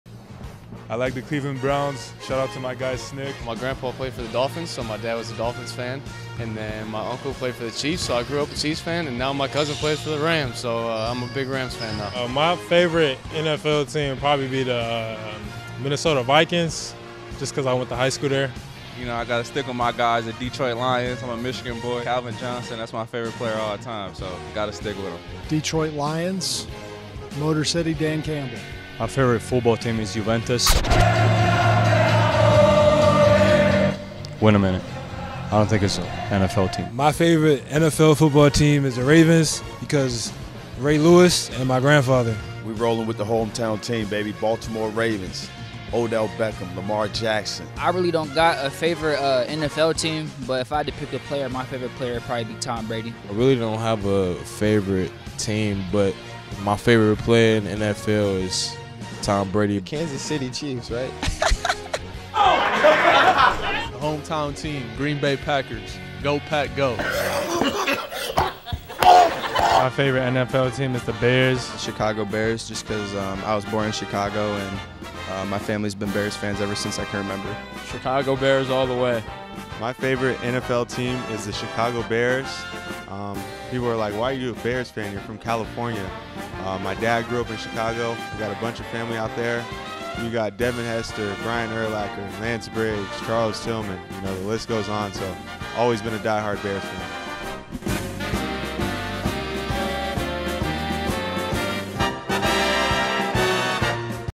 Illini Men's Basketball | Favorite NFL Team
When they're not donning their Illinois jerseys, you can catch our guys cheering for their favorite football teams.